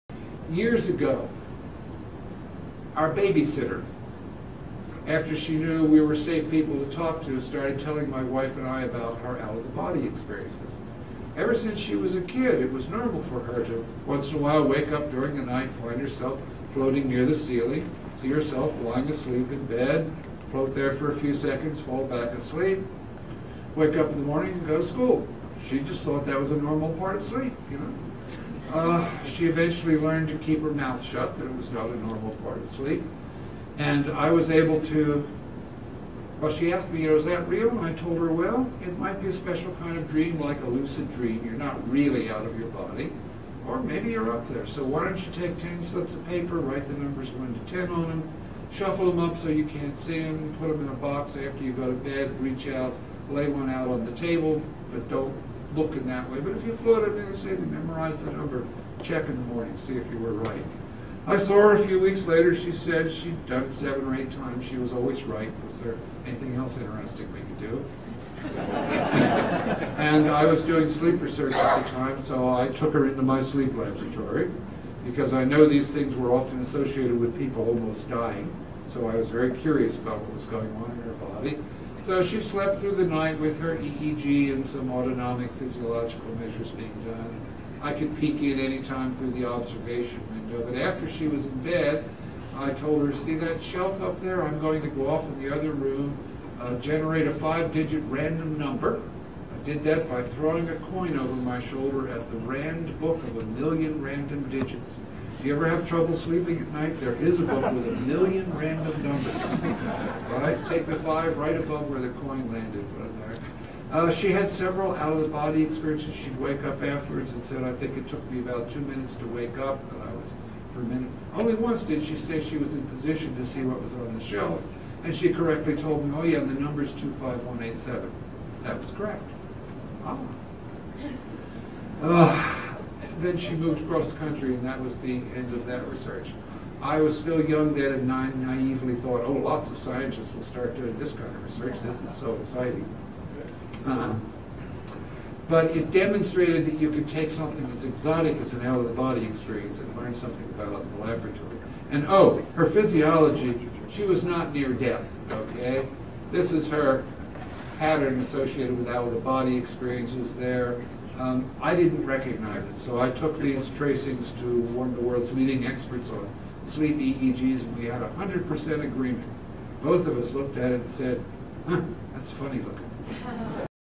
Charles Tart on OBEs at Buddhist Geeks 2014
Dr. Tart describes an experiment he did with a young woman who had regular out of body experiences.